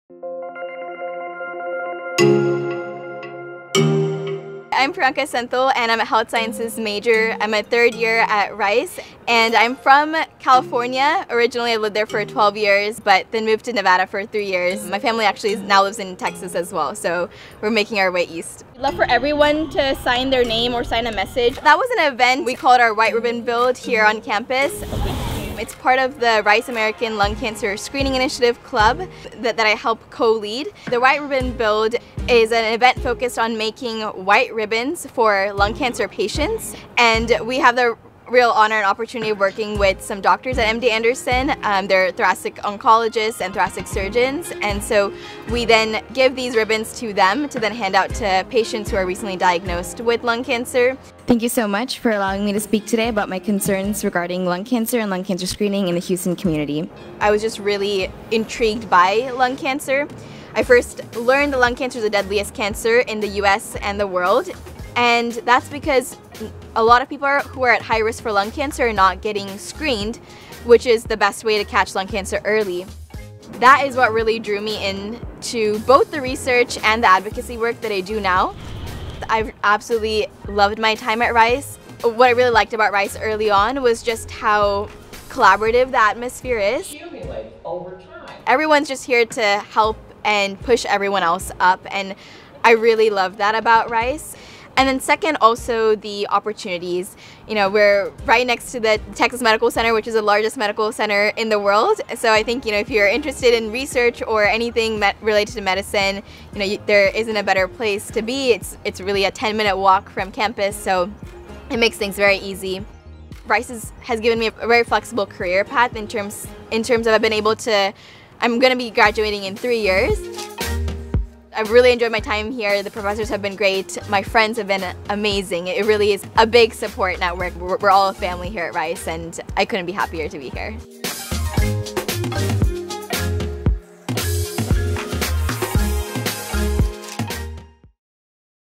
Unconventional Students at Rice: Priyanka Senthil advocates for lung cancer awareness
Priyanka Senthil is maximizing her opportunities at Rice University through her research and advocacy work related to lung cancer screening.

Senthil, a third-year student at Rice majoring in health sciences and minoring in medical humanities, has been working on clinical research related to improving lung cancer screening guidelines and treatment for the past three years.

“I’ve always been intrigued by lung cancer after I learned that it’s the deadliest cancer in the U.S. and the world,” Senthil said. “And that’s because a lot of people who are at high risk for lung cancer are not getting screened, which is the best way to catch lung cancer early. That’s what really drew me into both the research and advocacy work that I do now.”

Senthil is the president of ALCSI’s Rice chapter, which recently held a White Ribbon Build event where participants painted and signed wooden white ribbons for lung cancer patients.

“We have held over 490 community events and taught over 25,000 individuals about lung cancer screening,” she said. “We’ve also worked with mayors, governors and national leaders to issue proclamations and public service announcements encouraging constituents to get screened in addition to passing legislation around lung cancer screening.”

Senthil has published 10 papers related to lung cancer screening, including a first-author review paper that provides an update on the current lung cancer screening guidelines.

In March, she and her team published a study in the “Journal of Clinical Oncology” showing that using smoking duration, instead of pack-years, to determine lung cancer screening eligibility is more equitable and greatly increases opportunities for early lung cancer detection.

She also recently gave an oral presentation at the 2024 Academic Surgical Congress on a study regarding disparities in the surgical treatment of lung cancer between males and females in the United States. The study has found that females are more likely than males to receive inferior treatment for early-stage non-small-cell lung cancer.

Senthil said her research has made her passionate about finding ways to address underserved communities when it comes to accessibility of cancer treatment.

“I think it’s really important for groups to be doing research that highlights limitations and disparities in order to push for health policy change,” she said. “It goes back to trying to be a voice for people in communities that are oftentimes silenced or just not heard.”

Senthil’s time as a student at Rice has provided her with ample opportunities to pursue these passions boundlessly, she said.

“I have absolutely loved my time at Rice,” she said. “What I really liked about Rice early on was how collaborative the atmosphere is. Everyone is here to help and push everyone else up, and I really love that about Rice.

“Secondly, it’s the opportunities. We’re right next to the Texas Medical Center, which is the largest medical center in the world, so if you’re interested in research or anything related to medicine, there isn’t a better place to be.”